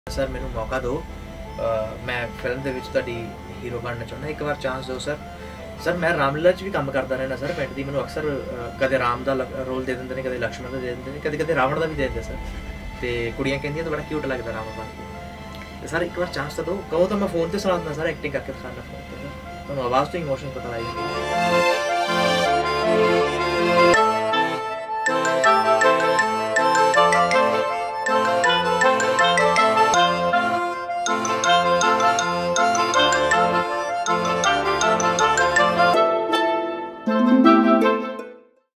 Teaser: Harish Verma on the sets of What the Jatt
Punjabi Mania interviewed Actor Harish Verma aka Jatt Tinka while he was shooting for his upcoming Punjabi movie What the Jatt. Harish talks about the movie and his character in it.